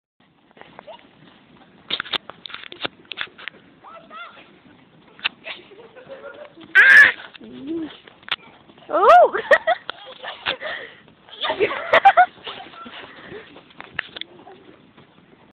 Alfie playing
Our little show pony playing follow the leader with me :) :)